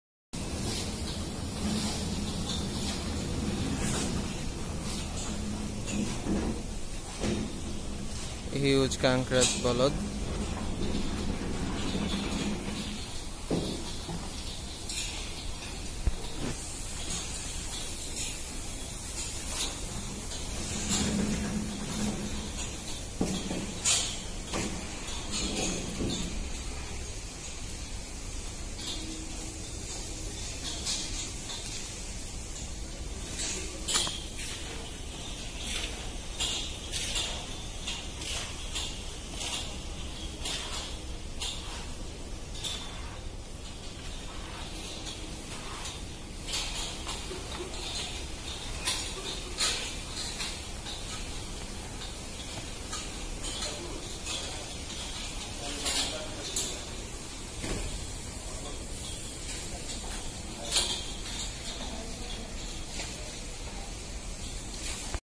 327 | Qurbani 2019 | Huge Kankraj Bolod | Rajesthani Special | Nabil Agro | ZbGH 2019
d'c studios presents no.327.

.Qurbani 2019 , Huge Kankraj Bolod from Rajesthani Special at Nabil Agro.